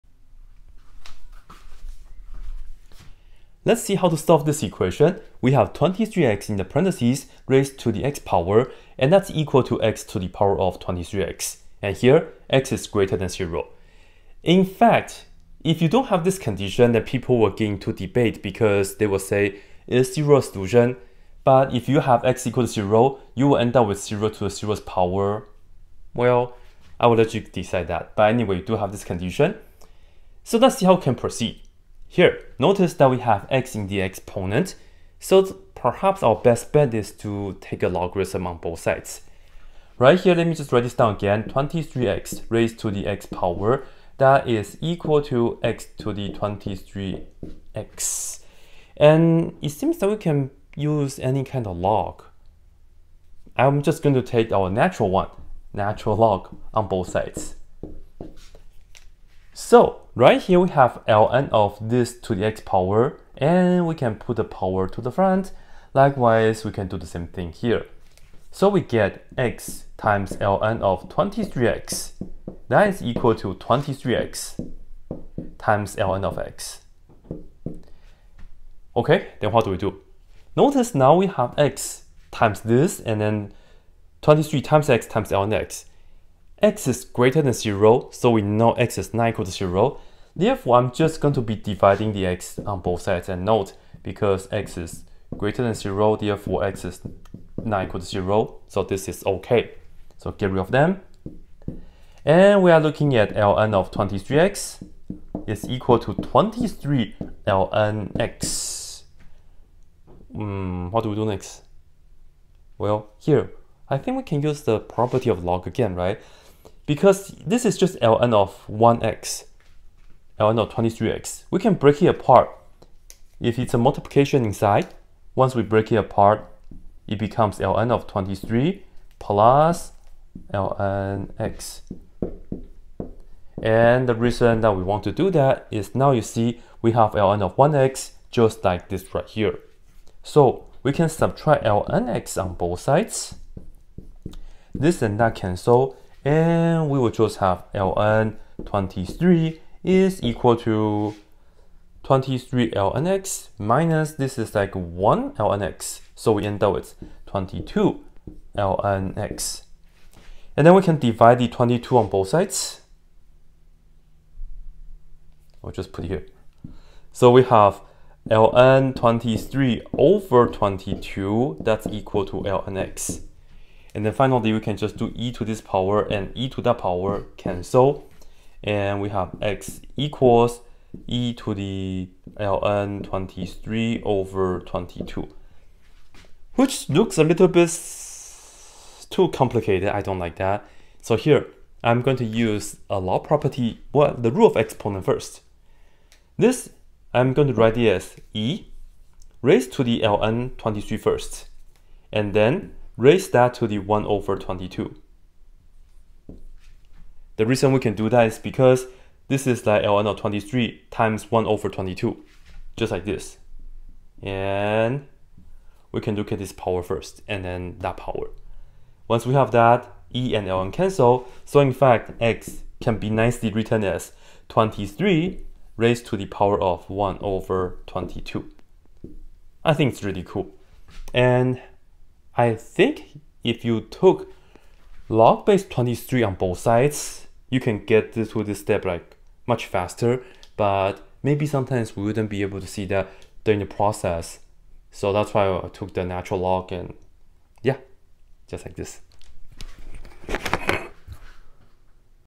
How do I find x? (23x)^x=x^(23x)
We will see how to solve the equation (23x)^x=x^(23x) by using logarithms. Try my most popular (1.9M views) exponential equation next